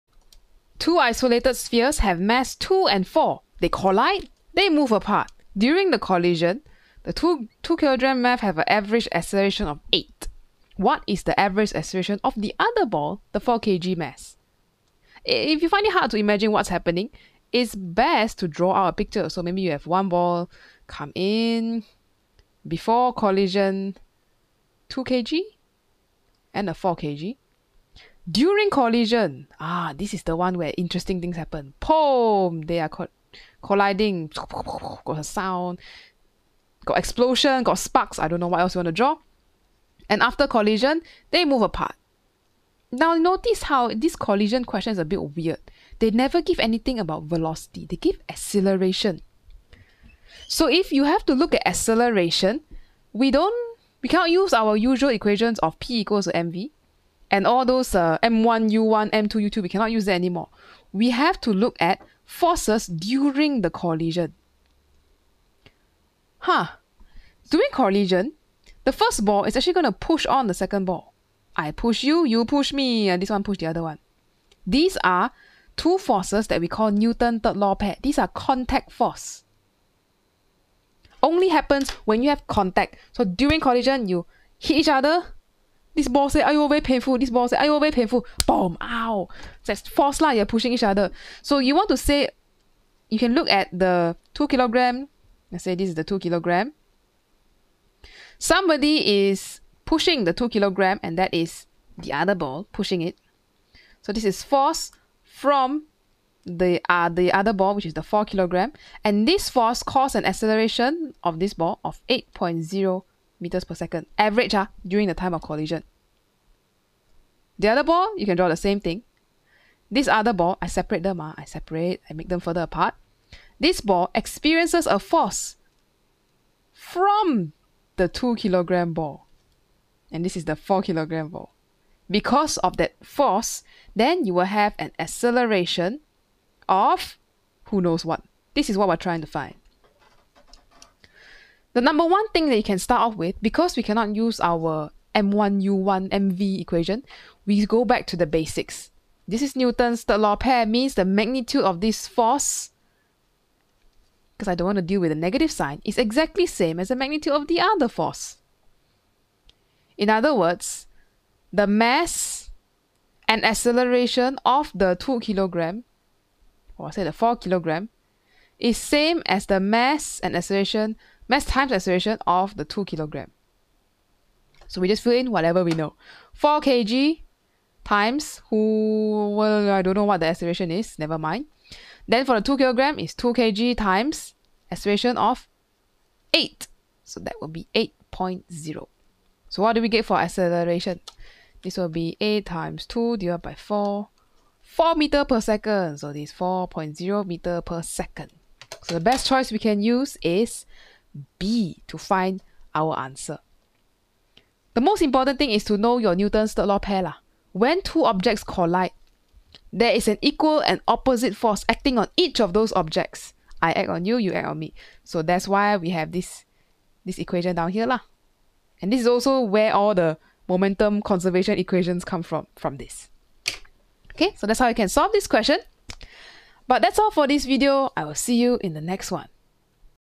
ON18 P13 Q7 Average Acceleration During Collision | Oct/Nov 2018 | Cambridge A Level Physics
9702/13/O/N/18: Two isolated spheres have masses 2.0kg and 4.0kg. The spheres collide and then move apart. During the collision, the 2.0kg mass has an average acceleration of 8.0ms–2. What is the average acceleration of the 4.0kg mass?